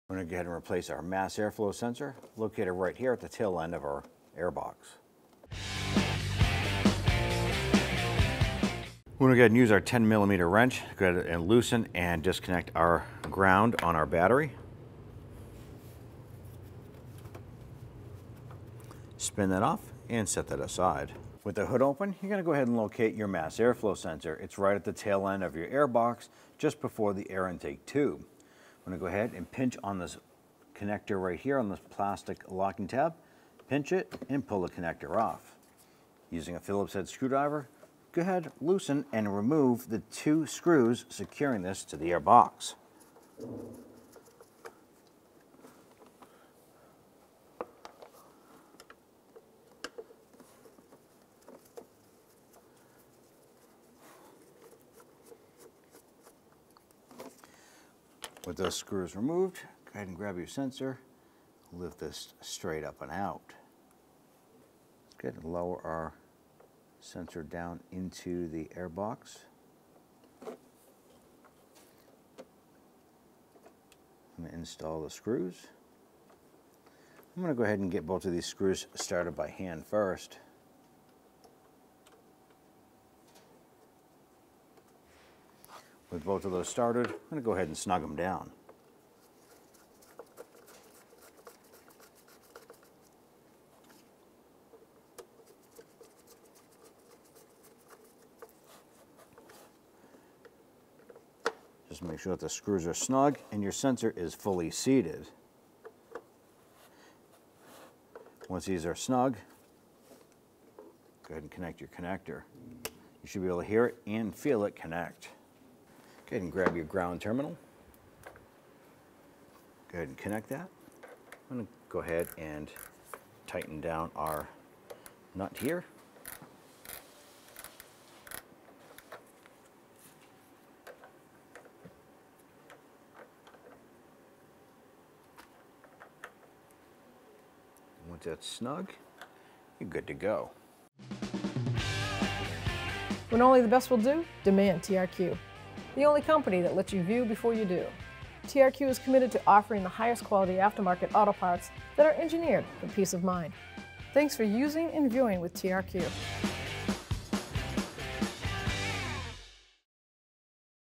How to Replace Mass Air Flow Sensor 2009-2015 Honda Pilot
This video shows you how to install a new TRQ MAF sensor in your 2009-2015 Honda Pilot. The Mass Air Flow (MAF) sensor determines the mass flow rate of air entering your vehicle. A trouble code indicating the sensor is not working properly could be because it is dirty, malfunctioning, or damaged, in which case your vehicle may run poorly or enter limp mode.  Fortunately, sensors are typically easy to replace.

This process should be similar on the following vehicles:
2009 Honda Pilot
2010 Honda Pilot
2011 Honda Pilot
2012 Honda Pilot
2013 Honda Pilot
2014 Honda Pilot
2015 Honda Pilot